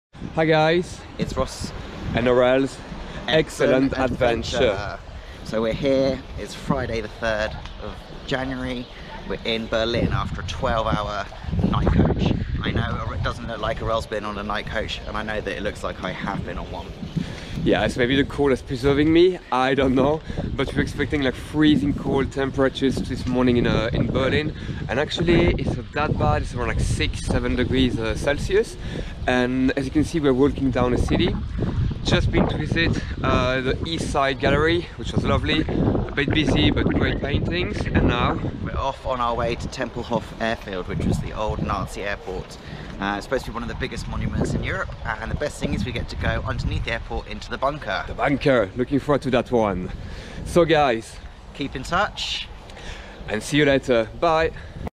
Ross & Aurel's Excellent Adventure - 1st stop Berlin!
In this video you get to see what 12 hours bus travel does for Ross' face and find out your first useless facts!

Want to keep up with our travels, look out for hashtag RAEA2020 on Instagram and Facebook